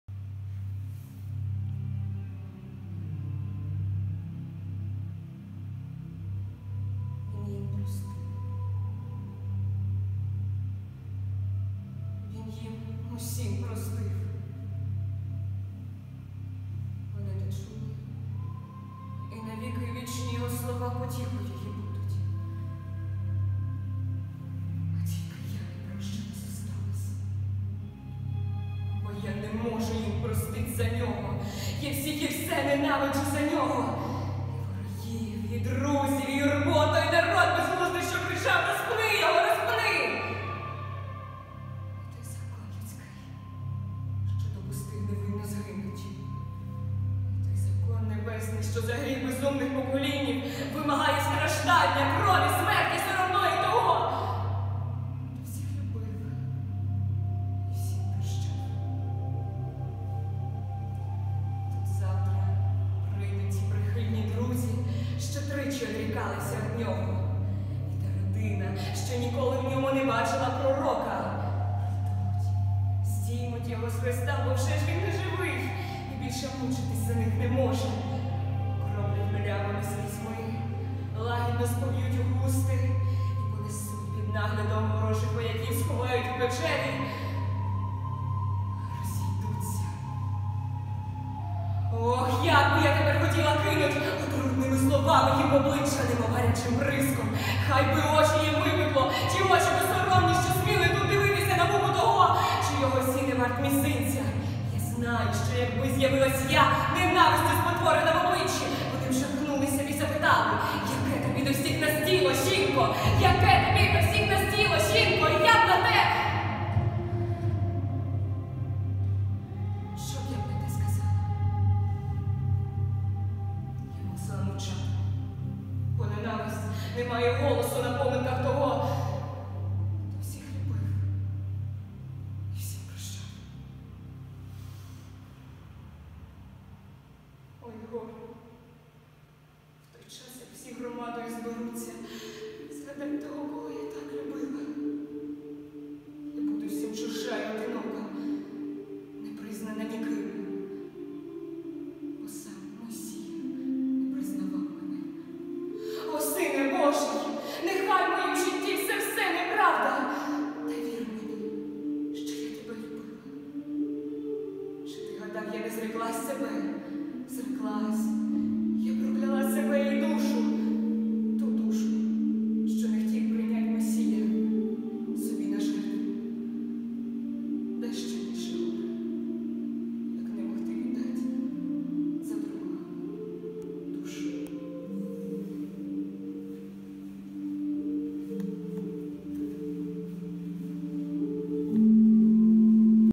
Орлова Маргарита "Одержима" | Конкурс «Змагаймось за нове життя!» 2022 рік
Номінація

Орлова Маргарита
"Одержима"
Вікова категорія: студенти

Підготувала - Місюра Олена Федорівна ,директор Будинку культури с.Доротище

Заклад - Володимир-Волинський педагогічний фаховий коледж ім. А.Ю Кримського

* Родини Бурбак.